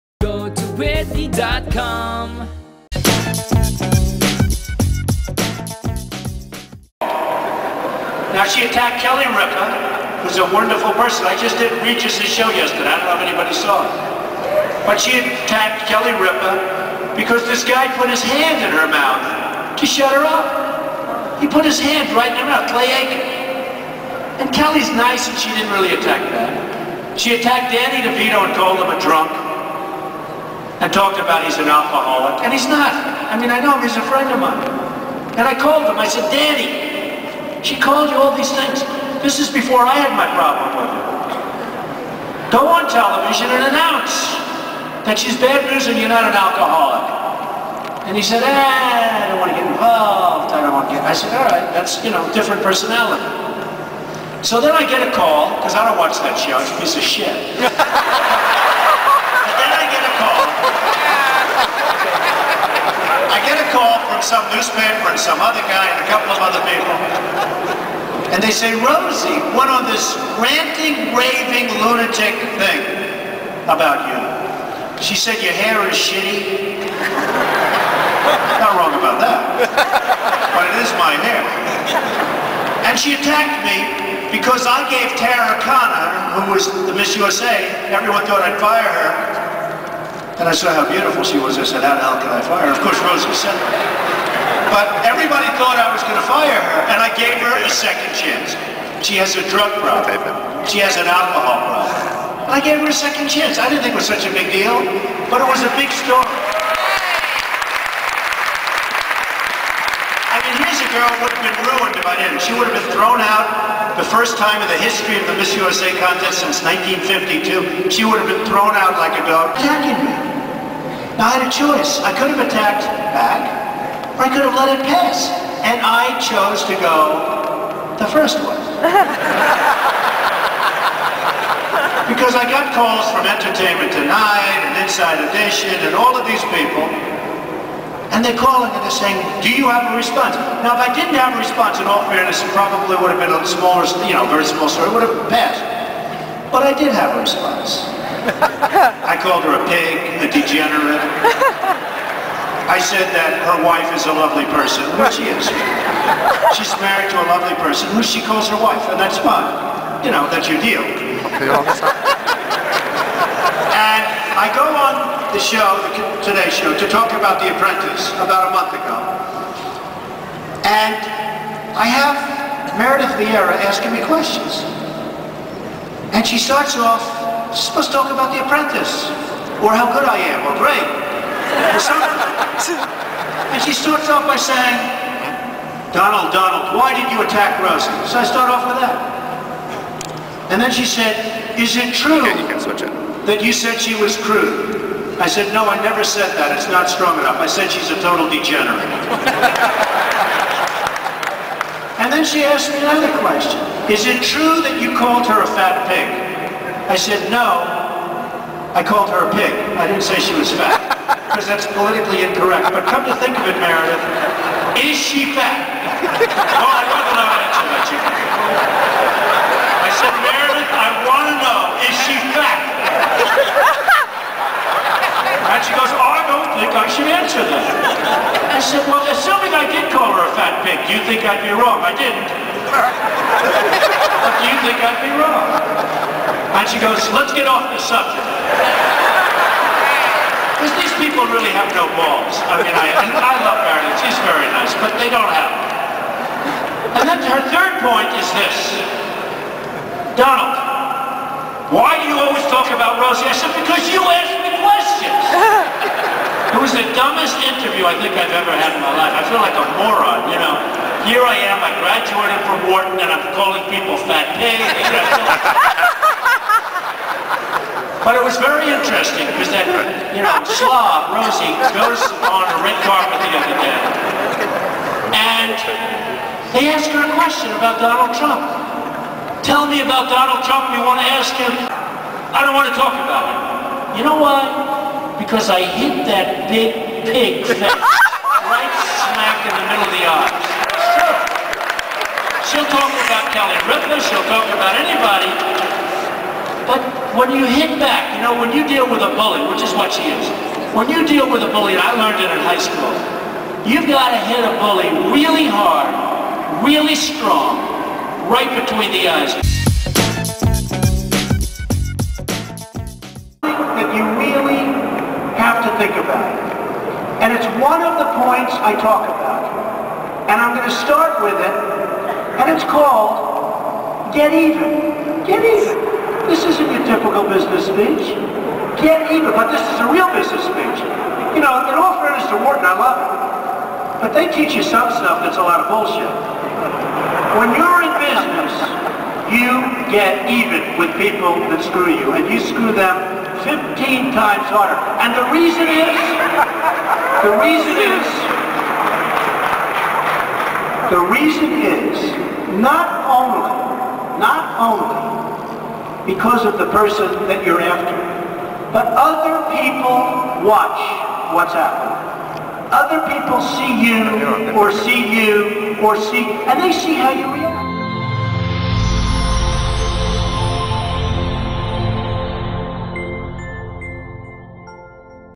Donald Trump on Rosie O'Donnell: She's a Pig, Degenerate & Slob "I Hate That Big Pig Face"
"I called her a pig, a degenerate, I said that her wife is a lovely person"

"I didn't say she was fat because that would be politically incorrect, but come to think of it...is she fat?"

"Here I am, I graduated from Wharton and I'm calling people 'Fat Pig'"

During a 2007 conference, Donald Trump tells the whole story about how Rosie O'Donnell attacks Kelly Ripa & Danny Devito and calls The View a piece of S**t. He also says that his interview with Meredith Vieira regarding the topic was probably the dumbest interview he's had in his life...

See all he has to say in the video.

This video was edited so many years ago, I wish we had the whole speech, but at least we have some of the good parts!!

Rosie O'Donnell
Kelly Ripa
Regis Philbin
Danny Devito
The View
Clay Aiken
Meredith Vieira